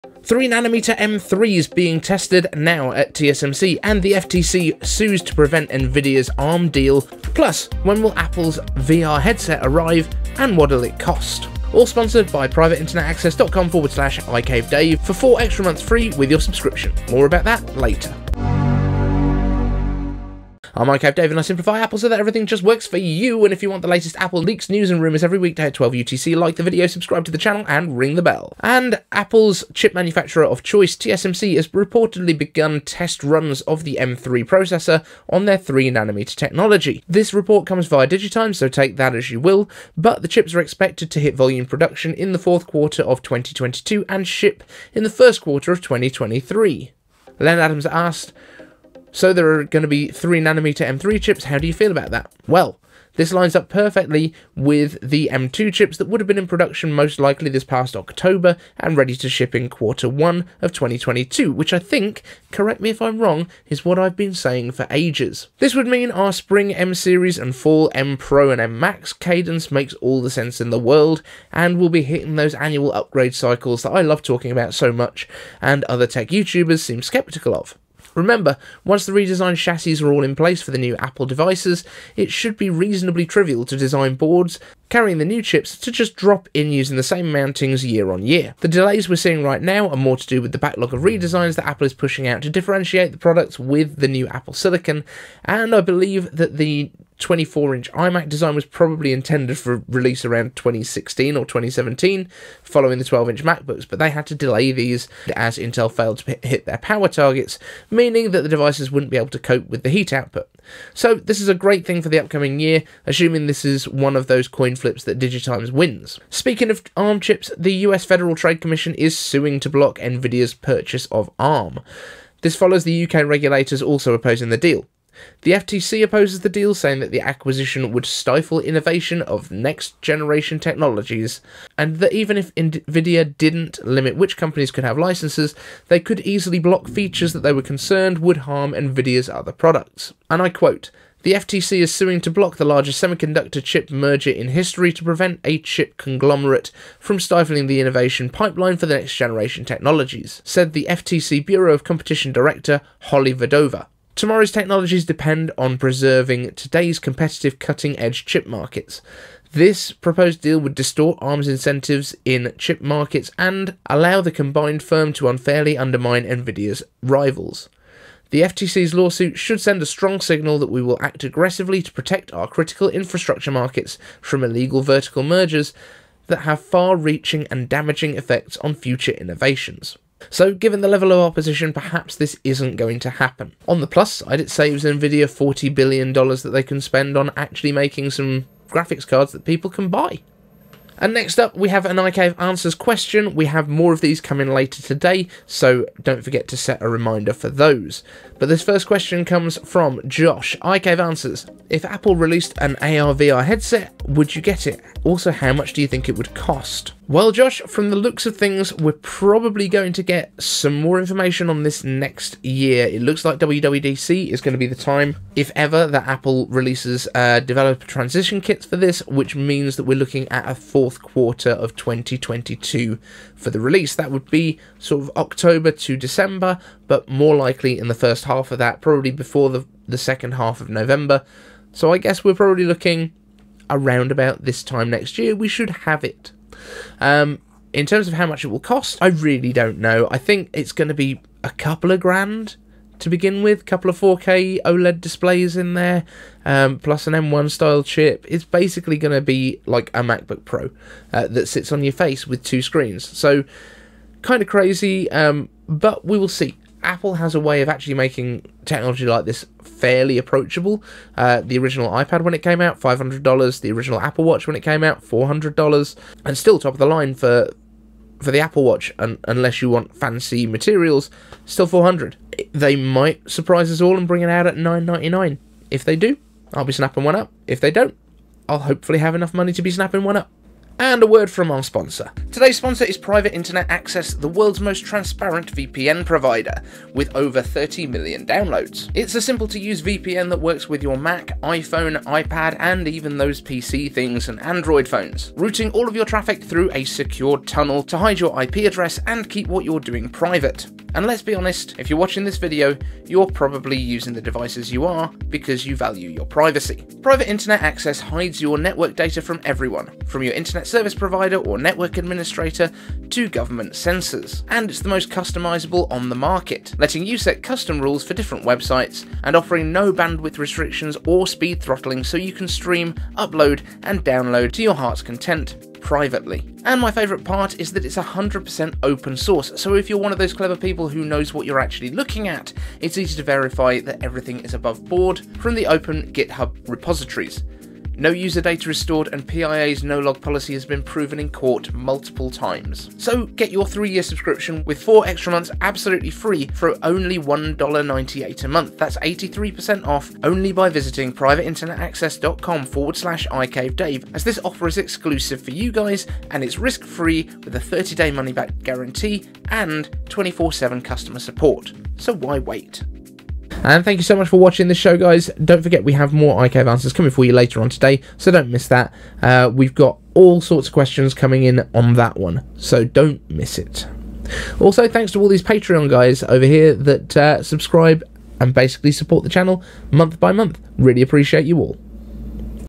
3nm M3 MacBook Chips being made at TSMC TODAY
3nm M3 being tested now at TSMC
FTC Sues to prevent Nvidia ARM deal
Apple VR/AR Headset release

Apple's chip manufacturer of choice, TSMC is reportedly begun test runs of the M3 processor on their 3nm technology. This report comes via Digitimes, so take that as you will, but the chips are expected to hit volume production in the 4th quarter of 2022 and ship in the first quarter of 2023.

Len Adams • 4 hours ago
iCaveAnswers so, there are going to be 3nm M3 chips How do you feel about that 🤔

This lines up perfectly then that the M2 chips would have been in production most likely this past October and ready to ship in Q1 of 2022 - which I think, correct me if I'm wrong, is what I've been saying for ages.

This would mean our Spring M series and Fall M Pro and M Max cadence makes all the sense in the world, and that we'll be hitting those annual upgrade cycles that I love talking about so much and other tech YouTubers seem skeptical of. Remember, once the redesigned chassis are in place for Apple's new devices, it should be reasonably trivial to design boards carrying the new chips to just drop in using the same mountings year on year.

The delays we're seeing right now are more to do with the backlog of redesigns that Apple is pushing out to differentiate their products with the new silicon, and I still believe that the 24" iMac design was probably intended for release around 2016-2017, following the 12" MacBook but had to be delayed and delayed as Intel failed to hit power targets, meaning that the devices wouldn't be able to cope with the heat output.

So this is a good thing for the upcoming year, assuming that this is one of those coin flips that Digitimes wins.

Speaking of ARM chips, the US Federal Trade Commission is suing to block Nvidia's purchase of ARM. This follows the UK regulators also opposing the deal. The FTC opposes the deal saying the acquisition would stifle innovative next generation technologies - and that even if Nvidia didn't limit which companies could have licences, they could easily block features that they were concerned would harm Nvidia's other products.

"The FTC is suing to block the largest semiconductor chip merger in history to prevent a chip conglomerate from stifling the innovation pipeline for next-generation technologies," said FTC Bureau of Competition Director Holly Vedova. "Tomorrow's technologies depend on preserving today's competitive, cutting-edge chip markets. This proposed deal would distort Arm's incentives in chip markets and allow the combined firm to unfairly undermine Nvidia's rivals. The FTC's lawsuit should send a strong signal that we will act aggressively to protect our critical infrastructure markets from illegal vertical mergers that have far-reaching and damaging effects on future innovations."

So given the level of opposition, perhaps this isn't going to happen.

Josh
iCaveAnswers if apple released an AR / VR headset, would you get it? Also how much do you think it will cost?